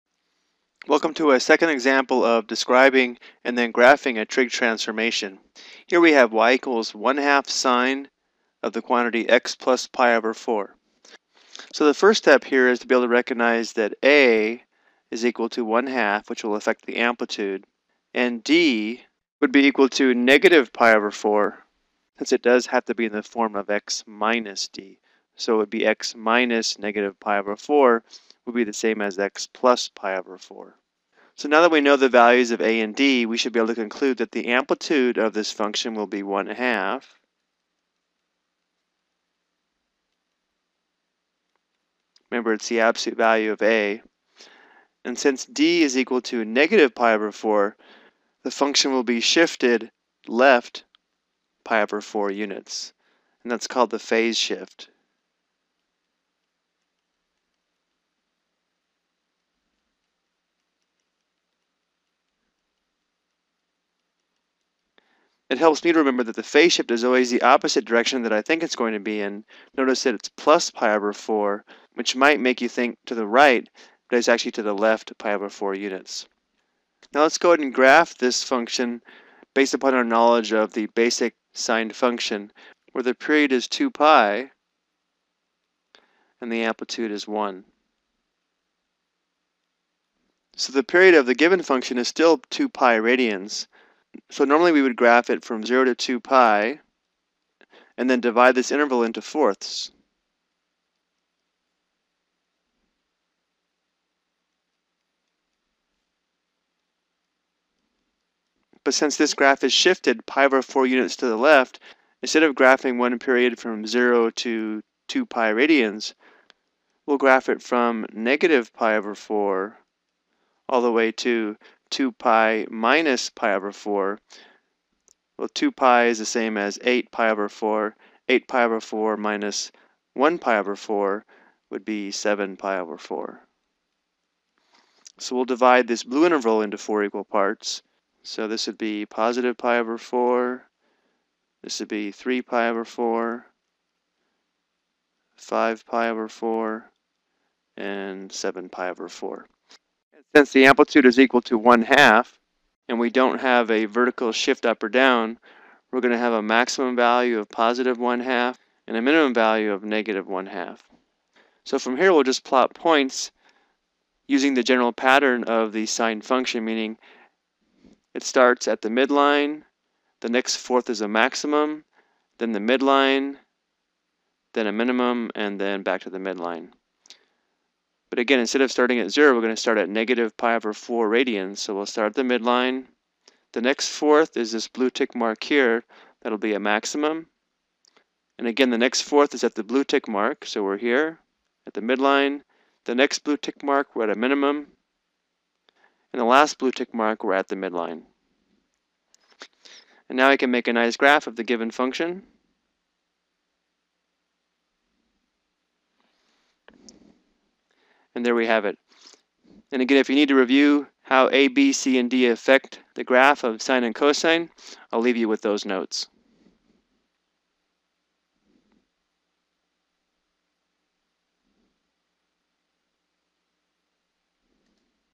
Example 2: Graphing a Transformation of Sine and Cosine
This video provides an example of describing and graphing a transformation of the sine and cosine functions.